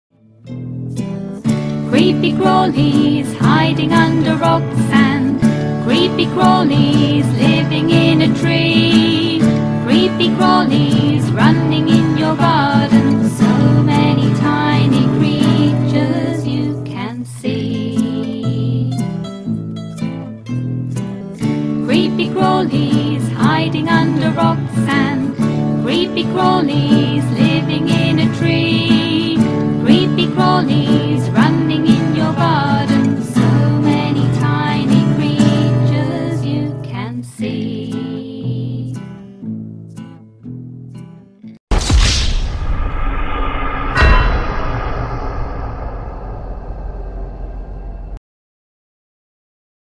Spotted Lanternfly early nymph stage INVASIVE SPECIES! MUST TAKE ACTION!
To learn more about these creatures (where they're from, what they're capable of) visit this website